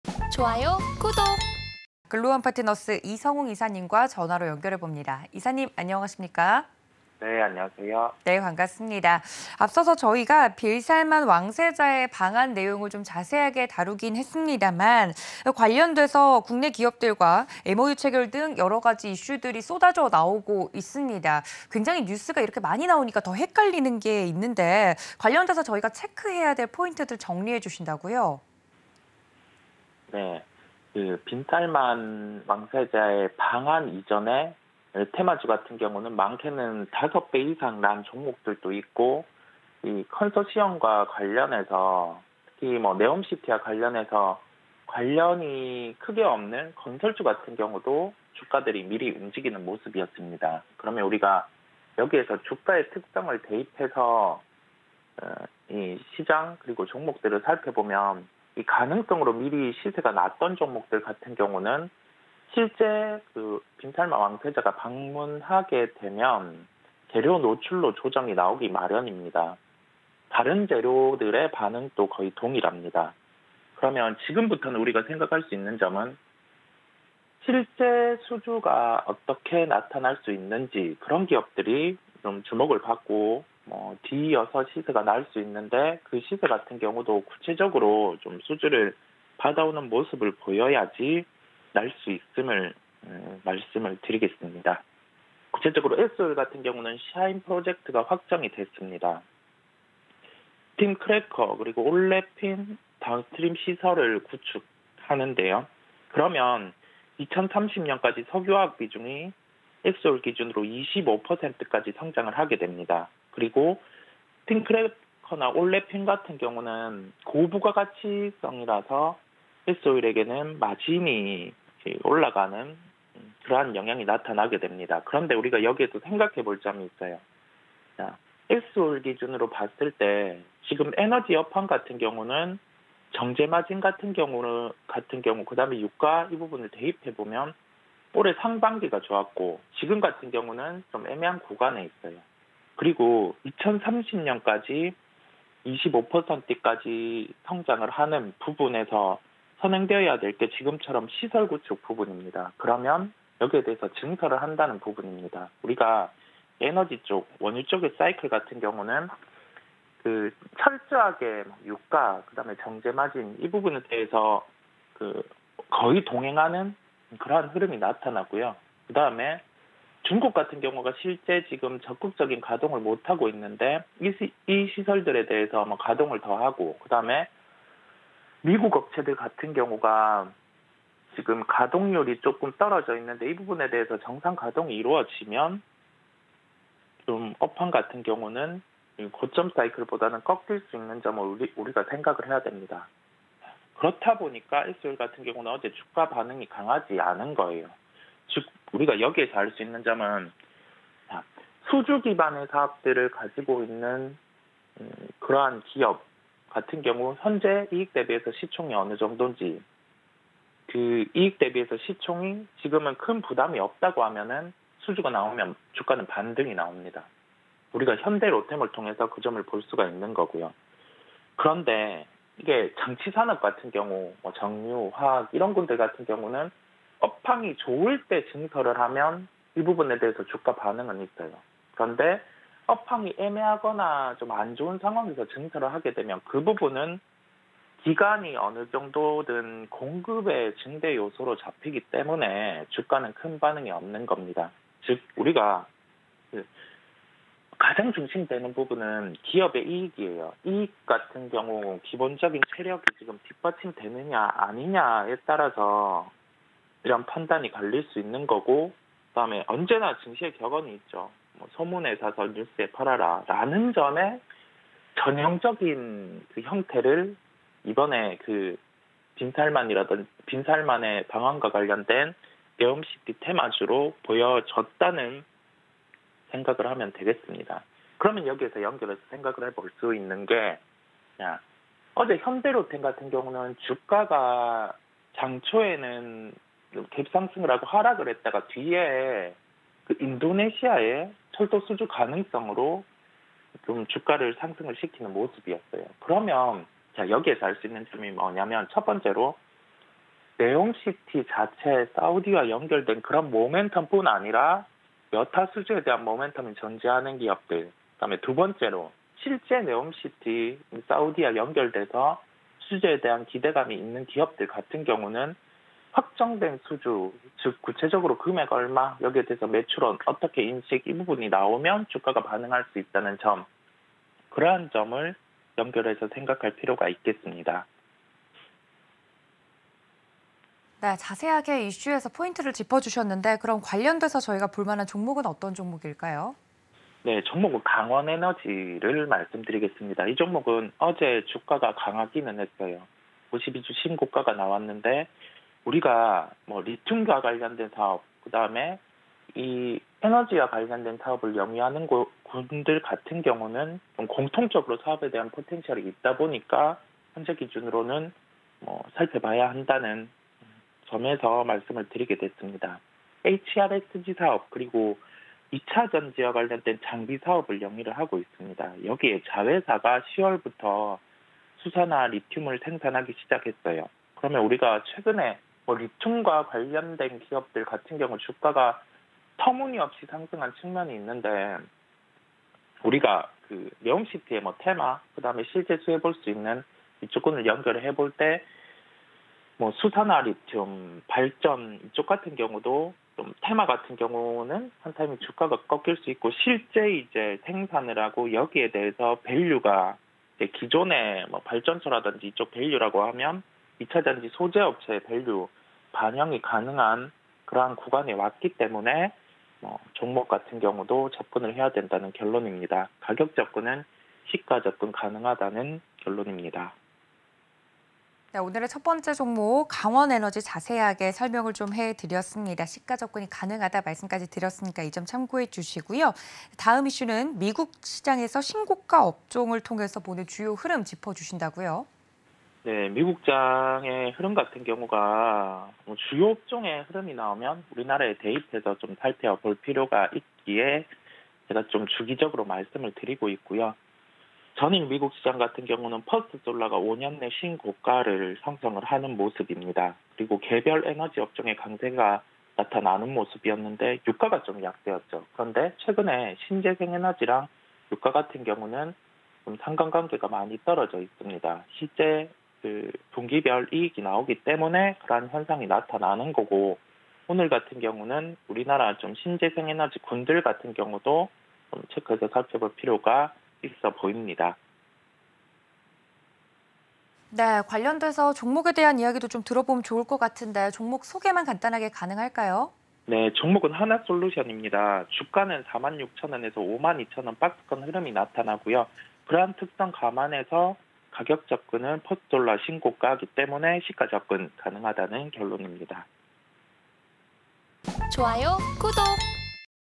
빈 살만, 국내 기업들과 MOU 체결..체크할 점은? │
-S-Oil의 샤힌 프로젝트 확정
-스팀크래커, 올래핀 다운스트림 시설 구축
2030년까지 석유・화학 비중 25%까지 성장
-증설에 대한 부분임을 감안
-빈살만 방한 관련 실제 계약 체결 여부 주목

이성웅 이사의 특징주 – 강원에너지
-HRSG 및 2차전지 장비사업
-자회사 수산화리튬 생산 시작
-2차전지 소재업체 밸류 반영 가능
-시가 접근 가능

미 시장 신고가 업종으로 보는 주요 흐름은?
-퍼스트솔라 5년래 신고가
-개별 에너지업종 강세

이성웅 이사의 특징주 – 한화솔루션
-분기 실적 연속 호조 가능
-46,000원~52,000원 박스권 흐름
-증설 단행시 주가 강세 예상
-시가 접근 가능

이 채널에서 더 많은 정보 받으실 수 있습니다~!